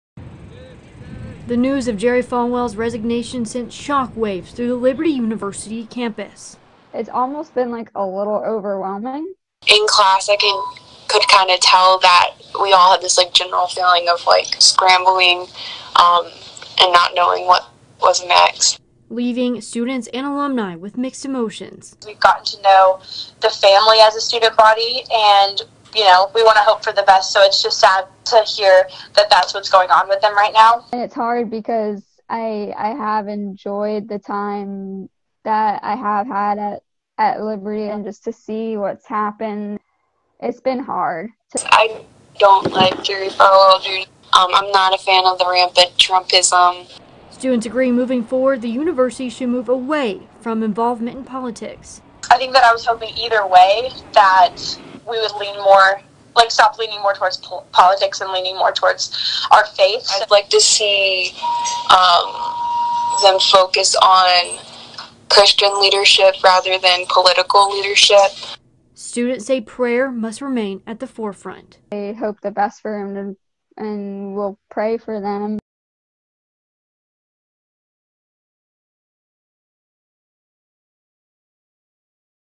Falwell says he's resigned from Liberty University
Jerry Falwell Jr. announced his resignation Tuesday as the head of Liberty University after a provocative photo and revelations of his wife's extramarital affair roiled the evangelical school founded by his father.